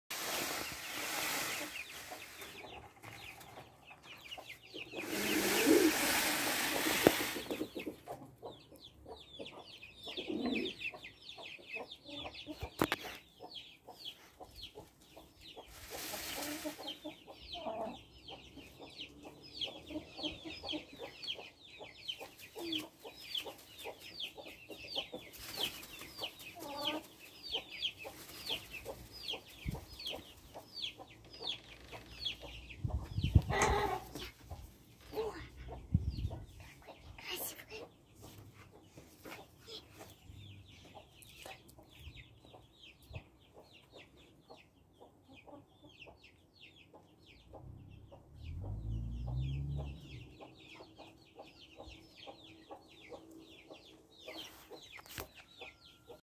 Павлин
Павлин распушил хвост